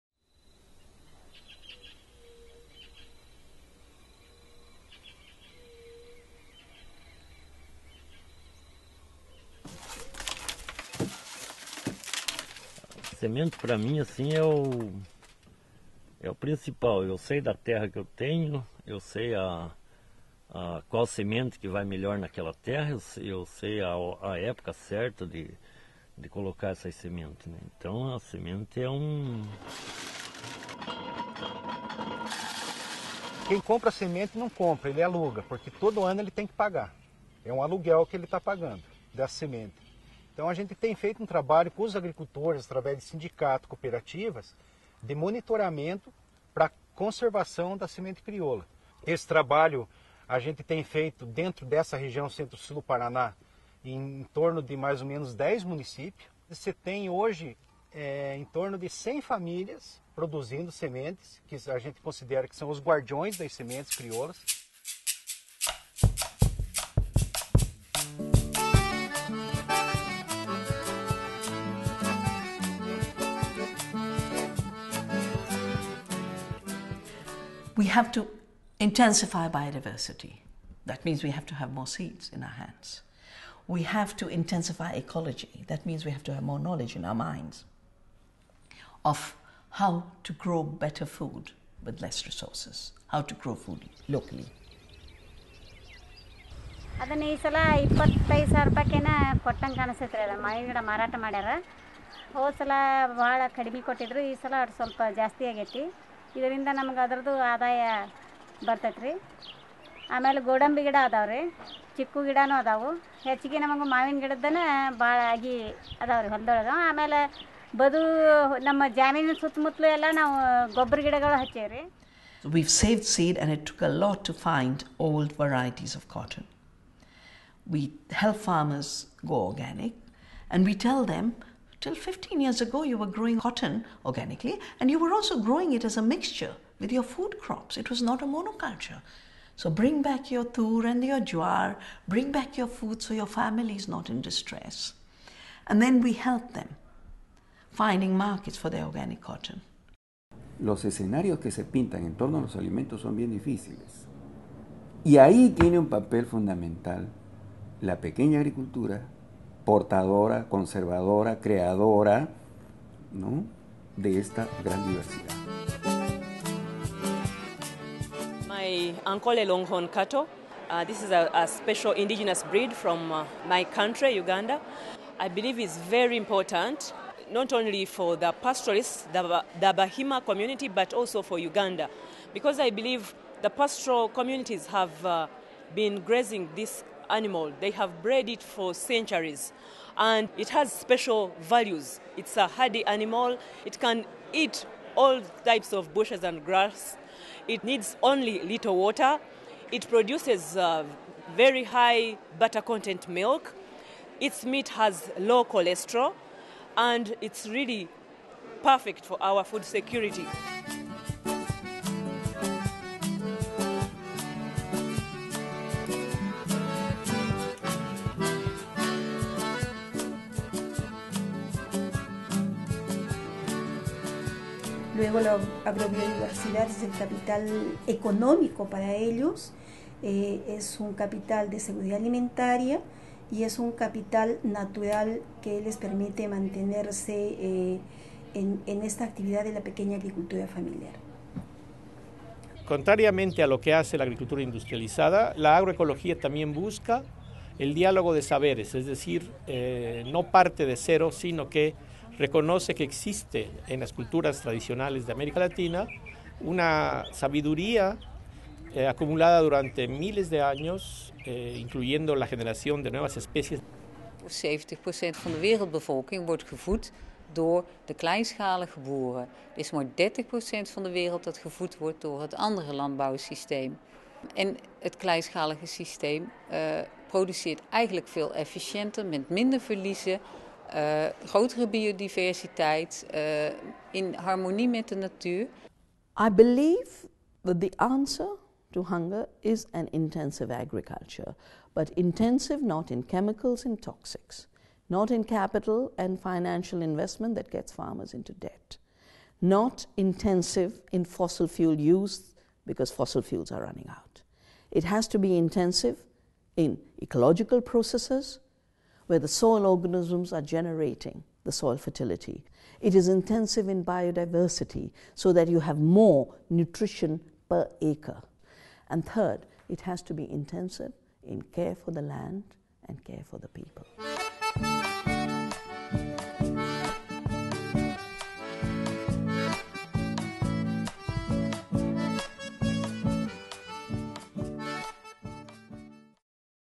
Biodiversity for food security and family farming
"I believe that the answer to hunger is intensive agriculture. Intensive not in chemicals, capital and fossil fuels, but rather intensive in ecological processes and biodiversity. In care for the land and care for the people." In five minutes, farmers and other experts such as Vandana Shiva and Victor Toledo explain why and how biodiversity is important for food security and for family farming.

This video was produced in combination with AgriCultures Network magazines 30.2 on agrobiodiversity,